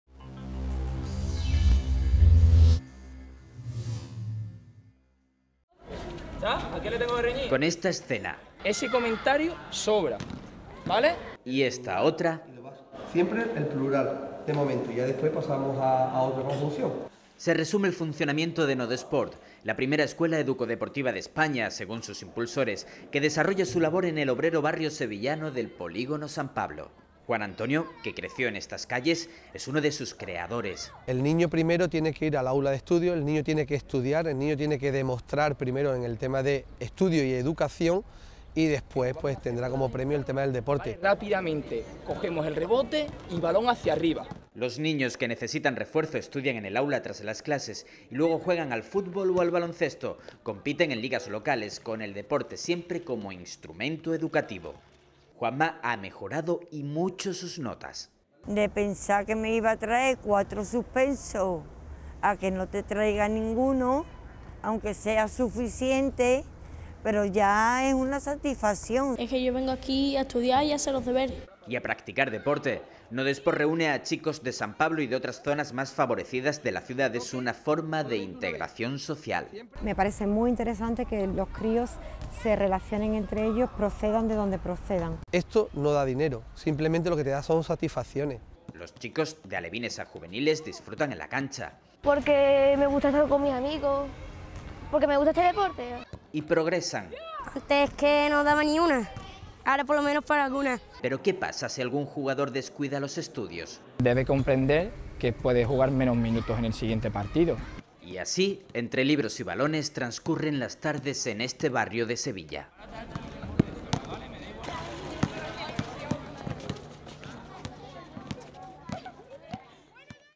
HDL Educadores crean la primera escuela que usa el deporte como incentivo para el estudio
Educadores sevillanos han creado la primera escuela educodeportiva que premia con el deporte a los estudiantes que cumplen con los objetivos académicos, además de llevar a cabo una labor de integración social ya que a ella acuden menores de distintos estratos. La escuela, Nodo Sport, se encuentra en un barrio obrero de Sevilla y ha sido puesta en marcha por dos educadores que se criaron en sus calles y que conocen las necesidades de una zona de estas características.